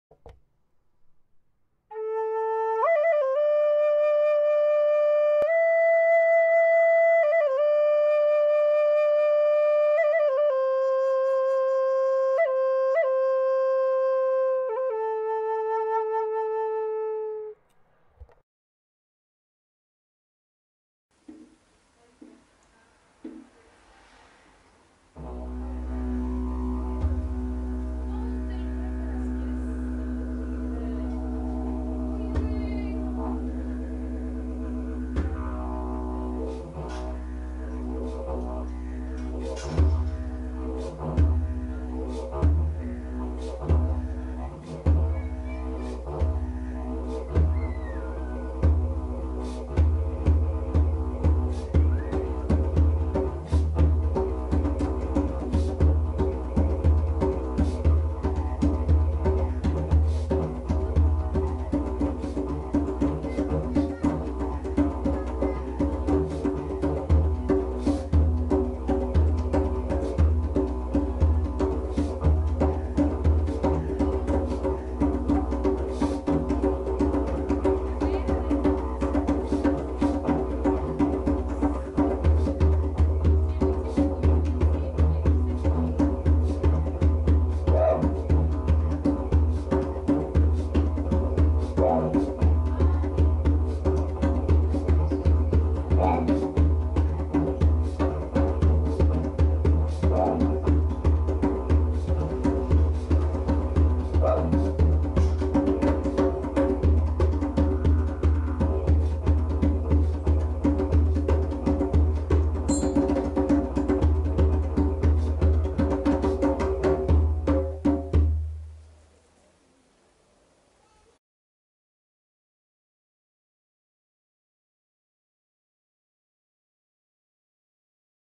Hom-Pax y Djembe
Extracto de un ensayo previo a un concierto ofrecido con los hermanitos de "Semillas de Poder" en la ciudad de San Bruno, California.